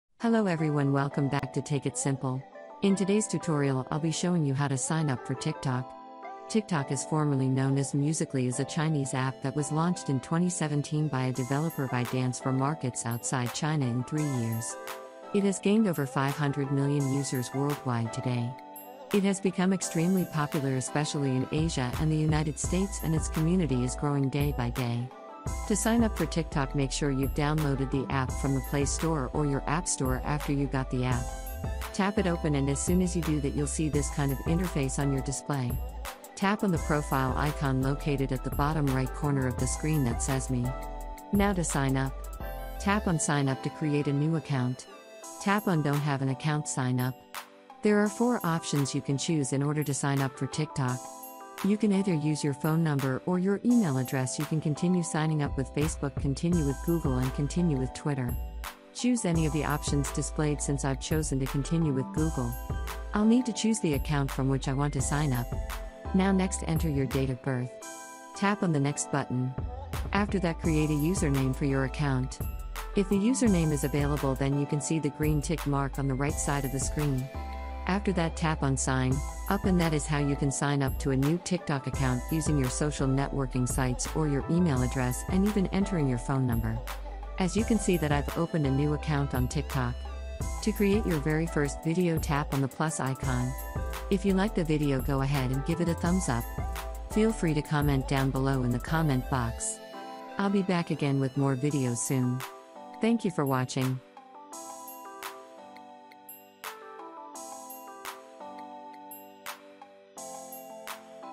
How to Create TikTok Account | TikTok Account Sign Up 2021
I'll be showing you how to sign up for TikTok.
 TikTok is formerly known as musically is a chinese app that was launched in 2017 by a developer by dance for markets outside china in three years it has gained over 500 million users worldwide today it has become extremely popular especially in asia and the united states and its community is growing day by day to sign up for TikTok make sure you've downloaded the app from the play store or your app store after you got the app tap it open and as soon as you do that you'll see this kind of interface on your display tap on the profile icon located at the bottom right corner of the screen that says me now to sign up tap on sign up to create a new account tap on don't have an account sign up there are four options you can choose in order to sign up for TikTok you can either use your phone number or your email address you can continue signing up with facebook continue with google and continue with twitter choose any of the options displayed since i've chosen to continue with google i'll need to choose the account from which i want to sign up now next enter your date of birth tap on the next button after that create a username for your account if the username is available then you can see the green tick mark on the right side of the screen after that tap on sign up and that is how you can sign up to a new TikTok account using your social networking sites or your email address and even entering your phone number as you can see that i've opened a new account on tiktok
Chapters
00:00  Intro
00:06  How to Create TikTok Account
01:33  Outro

Hey EveryOne!